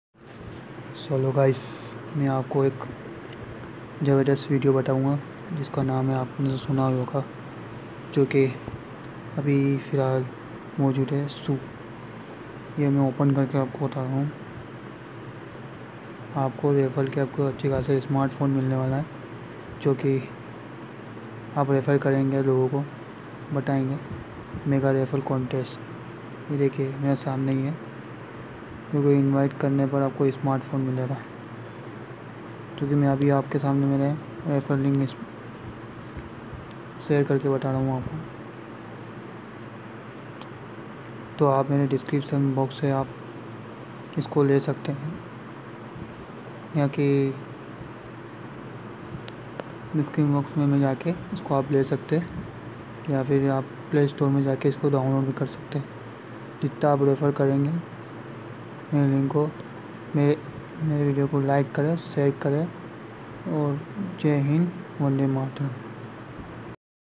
Swoo mega refferel contest for Smartphone win
Hello guy

Main refferel contest layo jo ke swoo lunch kiya ap jeet sakthay ek smartphone jo apna refferel code share karna bus ap meray nich diya refferel link use karay jaldi.